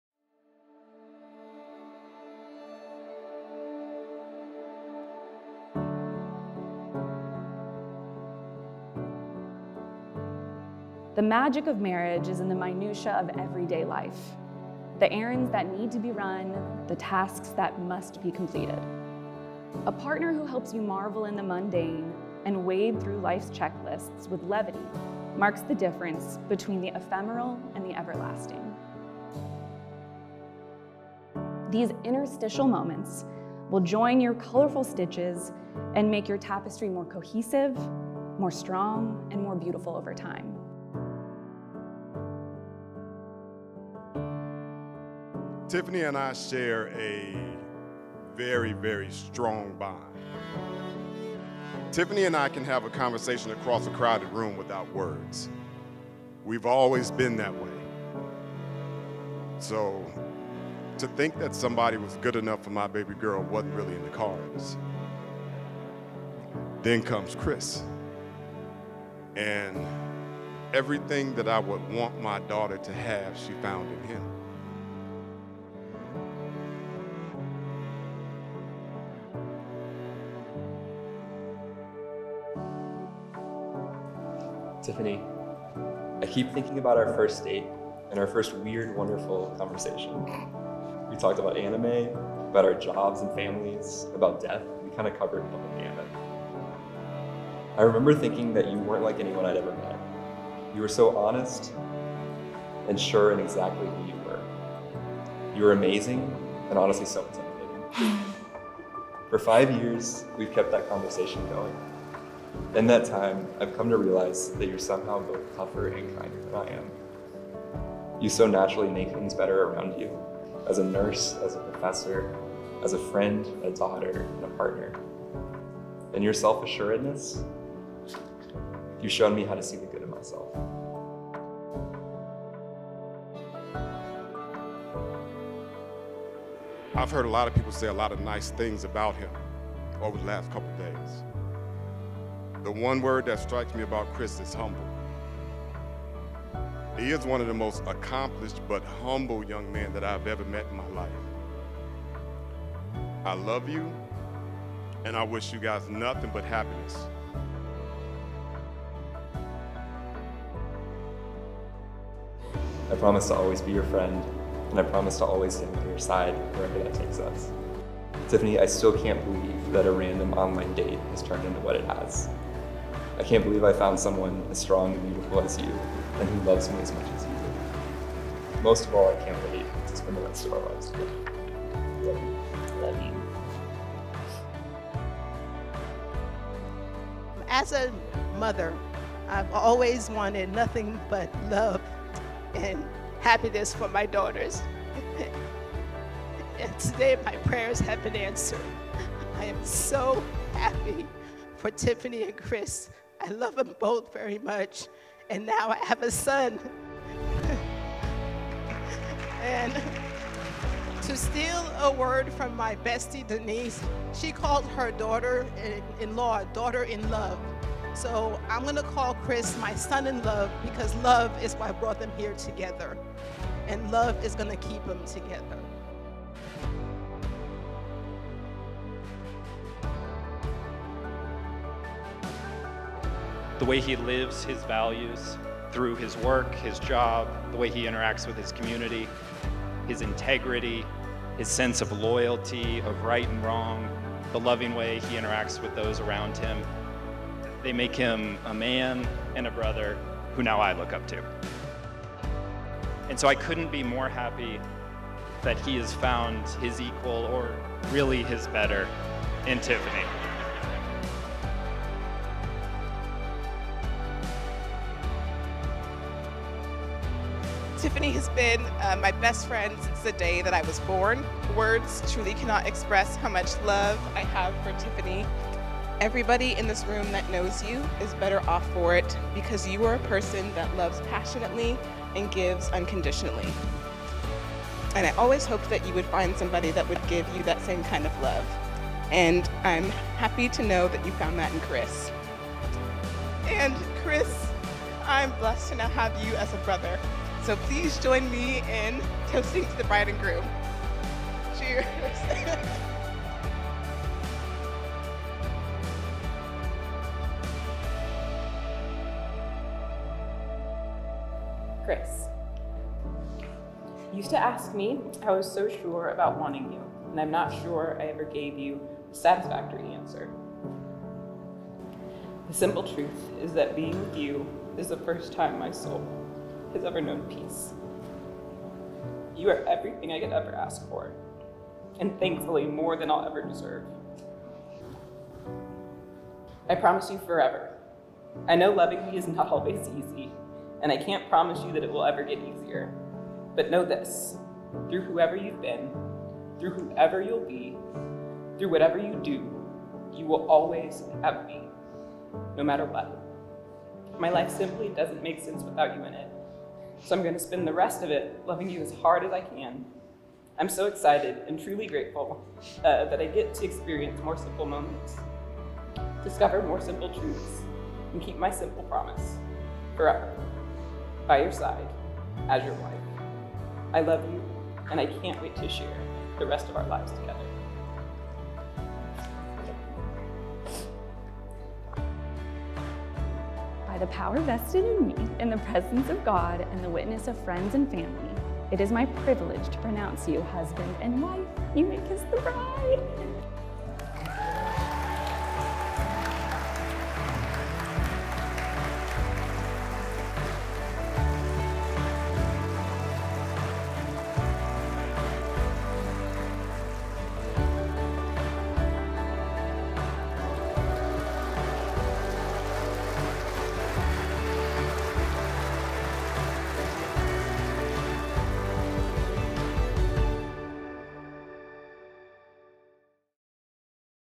Chris + Tiphany's Wedding at Ashton Gardens
"The magic of marriage is in the minutia of everyday of life. The errands that need to be run, the tasks that must be completed, a partner who helps you marvel in the mundane and wade through life's checklist with levity- marks the difference between the ephemeral and everlasting."

Congratulations to Tiphany and Chris, what a memorable day filled with so much love and happiness. The way that your family speaks about your spouse means THE WORLD, and to hear that these families fit so seamlessly and are so proud of each other really made us feel incredibly blessed to have witnessed the beginning of The Walters' legacy. Cheers to the newlyweds!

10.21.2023
Venue: Ashton Gardens Corinth
Photo: Sarah Boyd Photography
Video: VirtuReel Productions- VirtuWeddings
Cake: Jenny Layne
Car: Rolls Royce Concierge
Floral: Design Haus
DJ: CoHost Entertainment